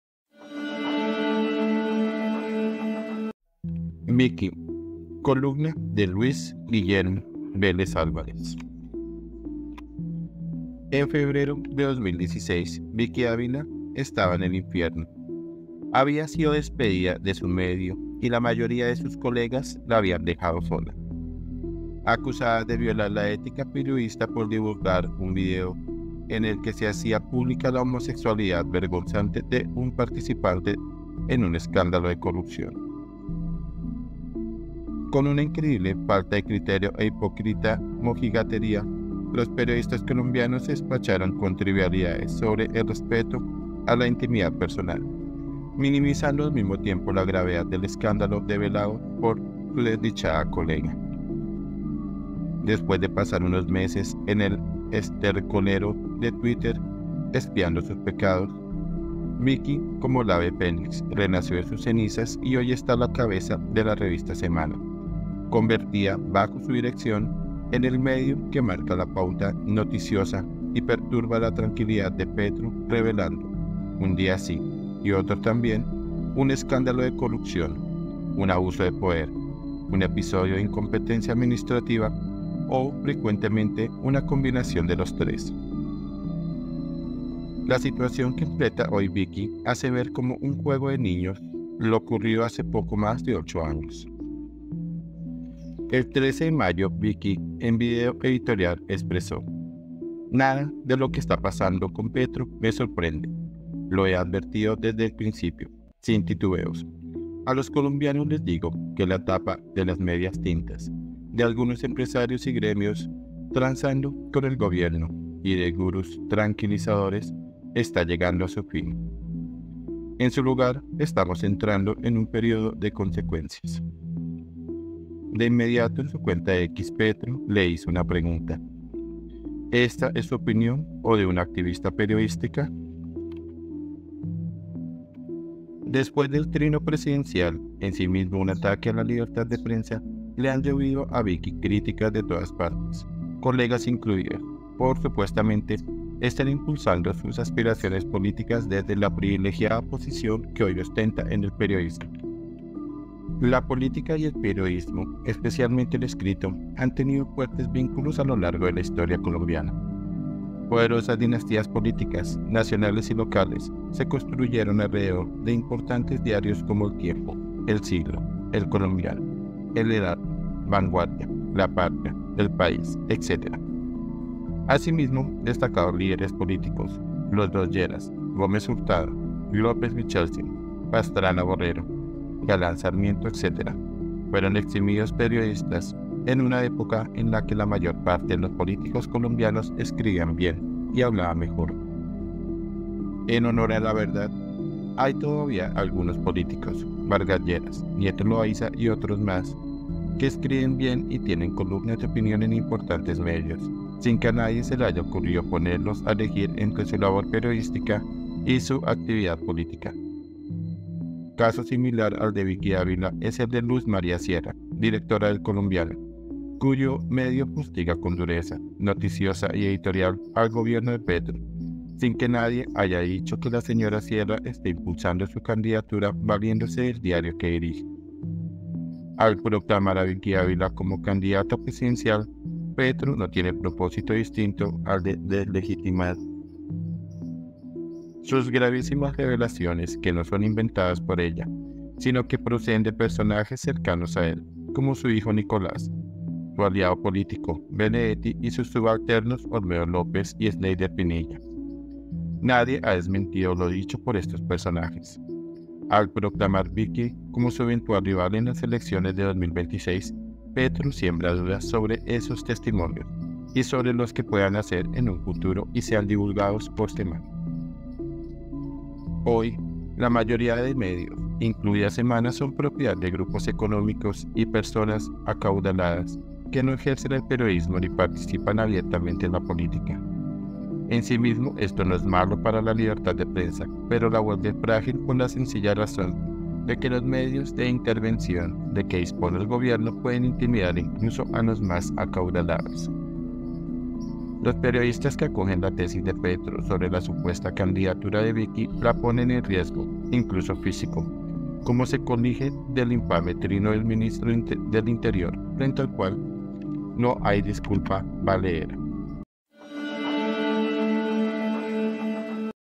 VICKY 🌐 Columna Luis Guillermo Vélez Álvarez
Los periodistas que acogen la tesis de Petro sobre la supuesta candidatura de Vicky, la ponen en riesgo, incluso físico, como se colige del infame trino del Ministro del Interior, frente al cual no hay disculpa valedera.
columna LUIS GUILLERMO VÉLEZ ÁLVAREZ